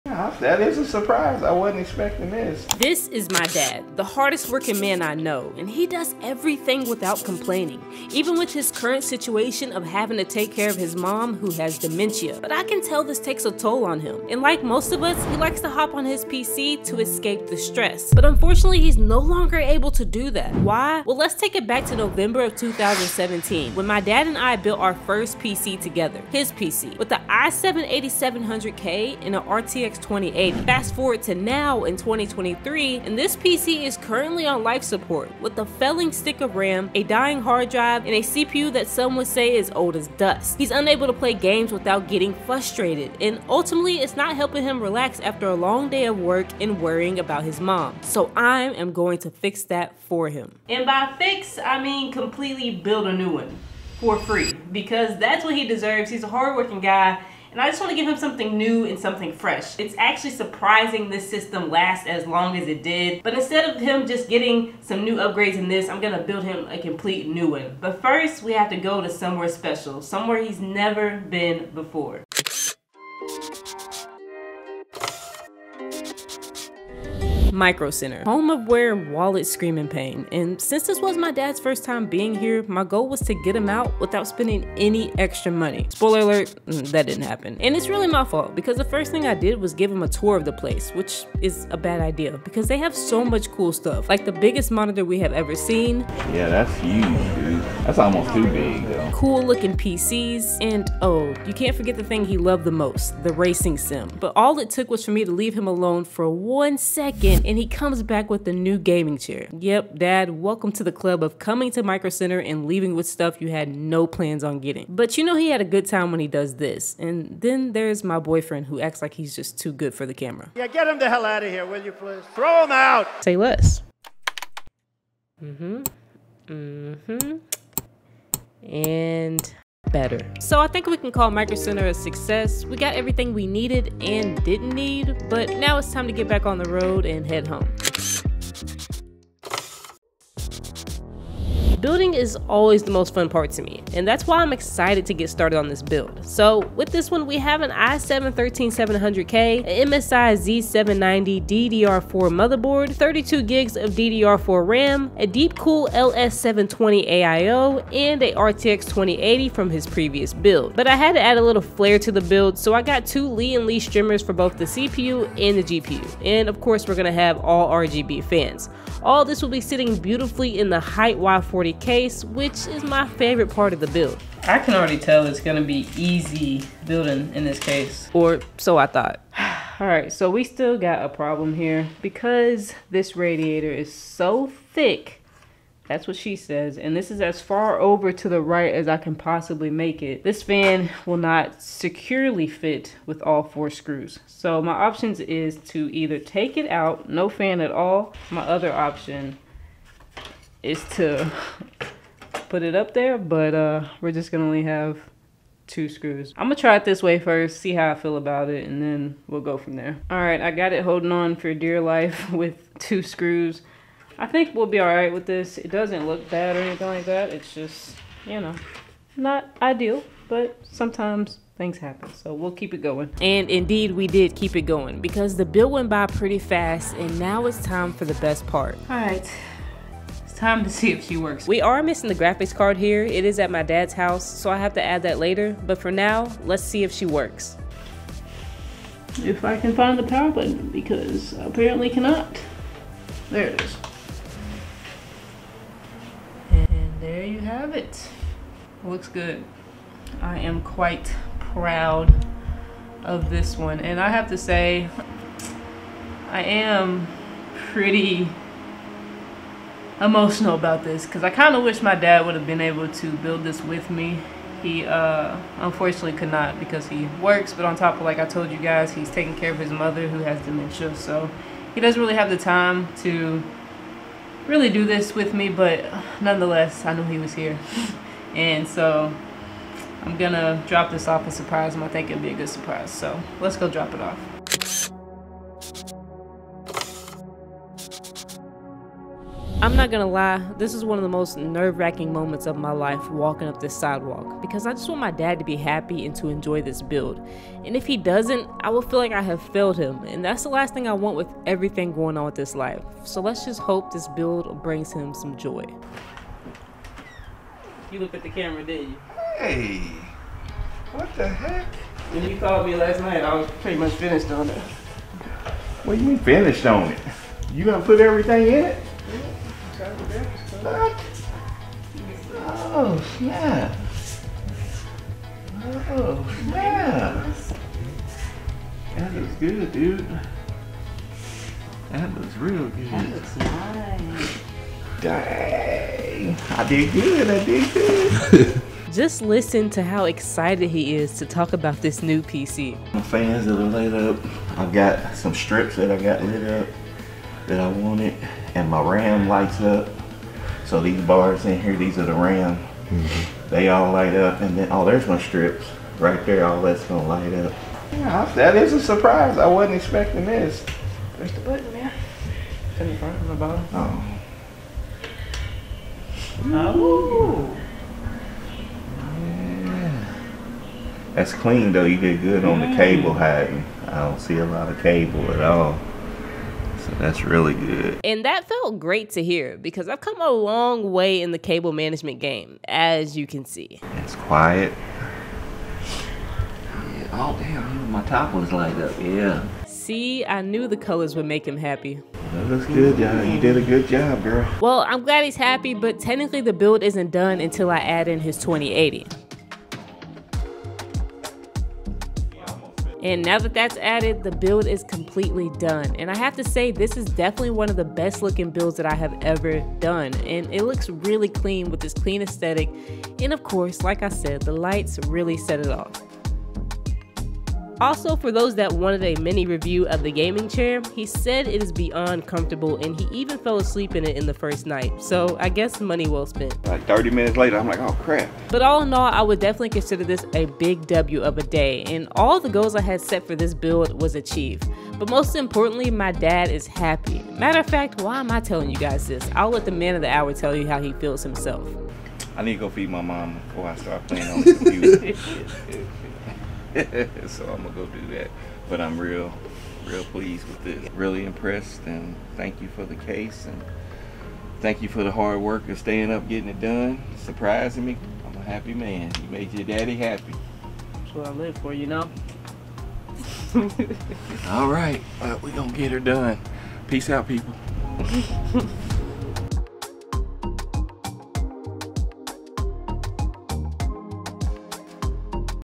You won’t believe how my Dad reacted when I surprised him with this…
My Dad takes care of his mother with dementia every day. He is stressed out and needs an escape. His old pc was that escape until it no longer worked correctly. I could no longer watch him suffer so I surprised him with a new PC!

PC Parts:
i7 13700k
MSI Z790 Motherboard
32 GB DDR4 3600 mhz RAM
1 TB Samsung 980 Pro SSD
Lian Li Strimmer for CPU and GPU
DeepCool LS720 AIO
DeepCool 120mm RGB Fans
Hyte Y40 Case

Gear:
Camera: Canon R10
Lens 1: 15-35mm RF lens
Lens 2: 35mm RF lens
Lens 3: 16mm RF lens
Elgato Wave 3 Mic